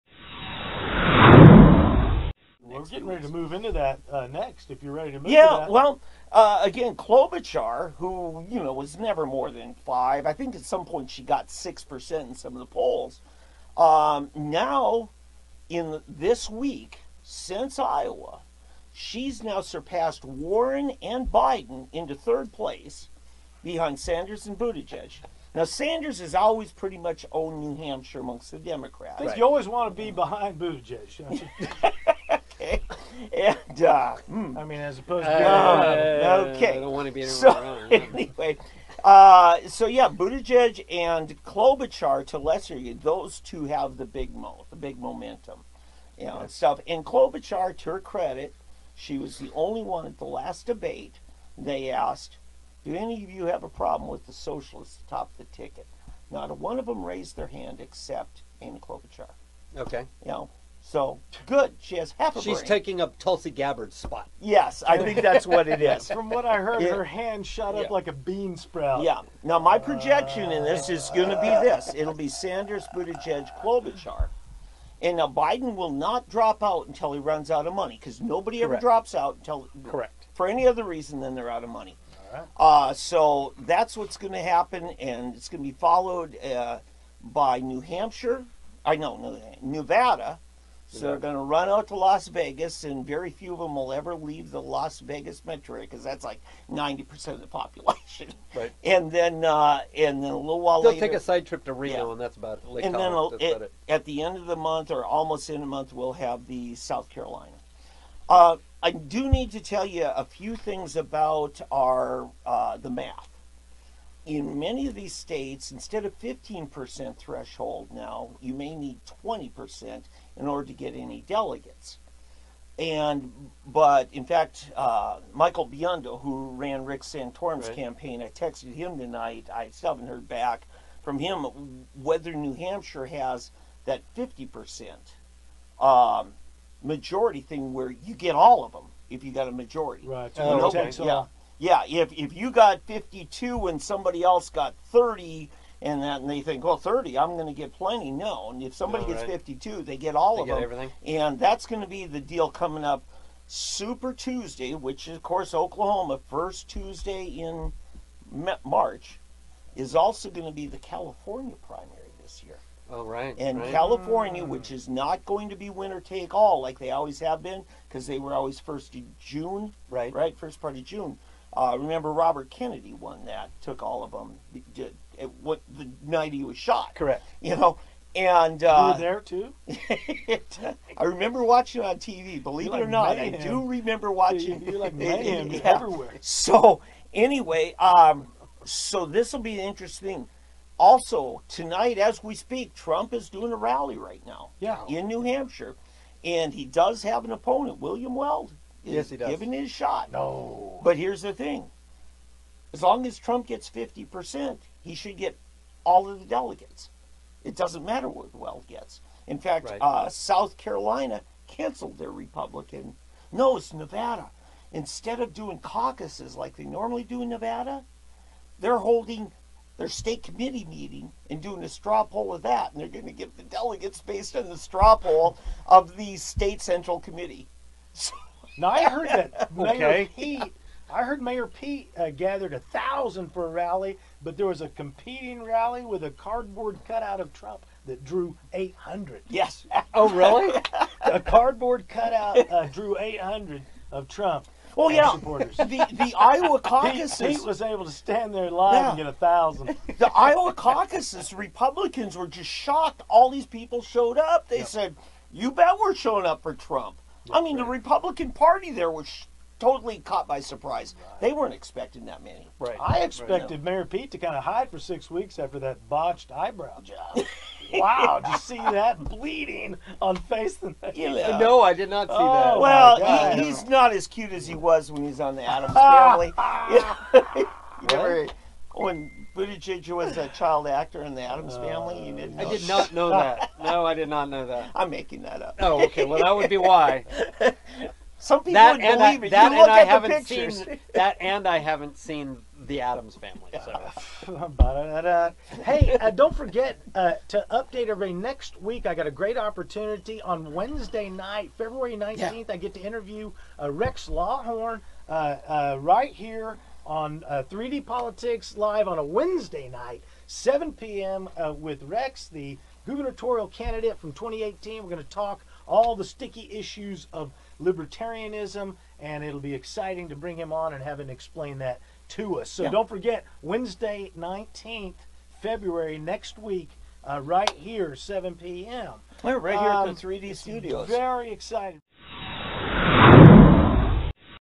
Primary Update For Feb 10th
In this installment of 3D Politics Live..
-  Iowa just gave 'participation trophies', until it didn't matter. But New Hampshire lets the state election board run the election. Let's see if the 'Liberty State' can give us more clarity about who's ascending and who's declining ("Hello, Joe!")

- Watch the entire weekly telecast live, every Monday night at 7pm, on our 3D Politics Facebook